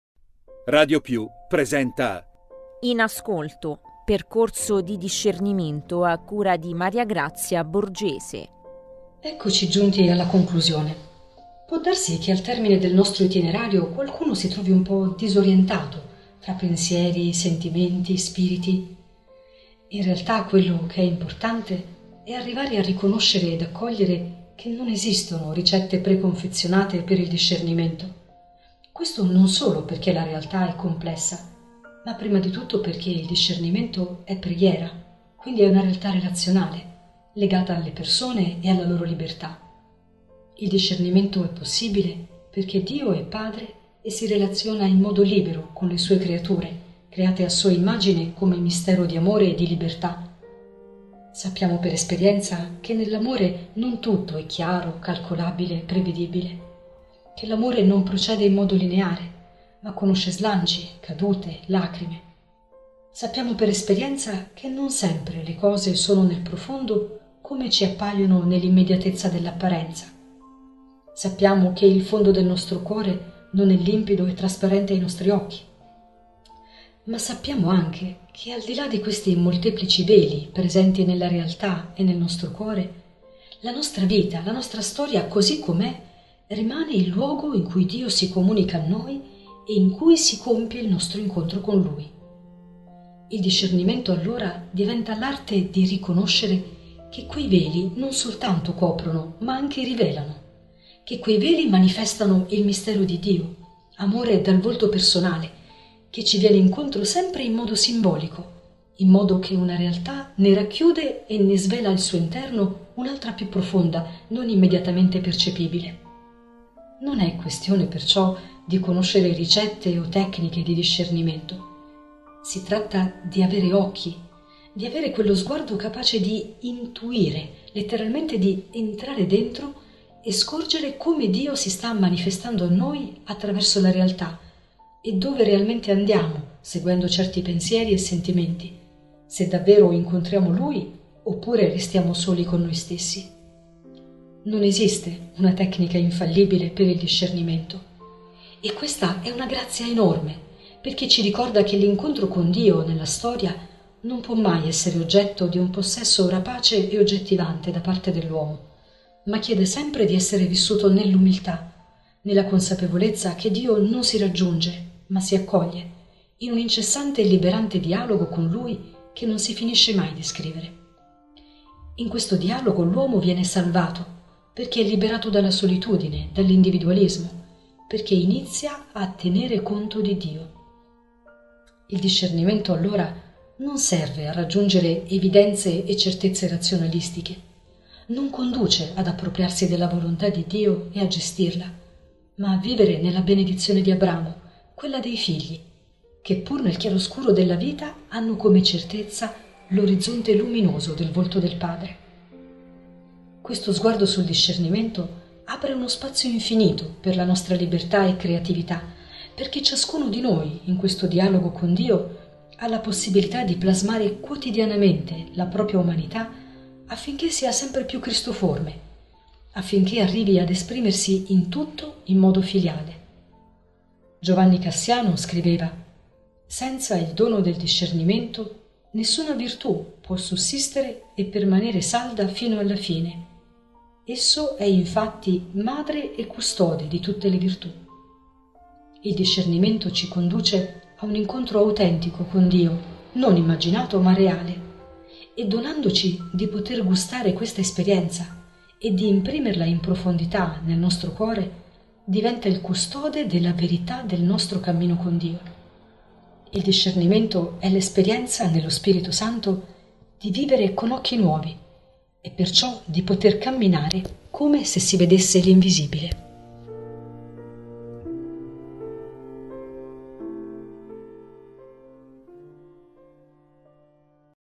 In ascolto - Vedere l'invisibile
Vedere l’invisibile. Non esiste una tecnica infallibile per il discernimento. Lo sguardo sul discernimento apre uno spazio infinito per la nostra libertà e creatività perché ciascuno di noi, in questo dialogo con Dio, ha la possibilità di plasmare quotidianamente la propria umanità affinché sia sempre più simile a Cristo. La riflessione a cura della professoressa Maria Grazia Borgese.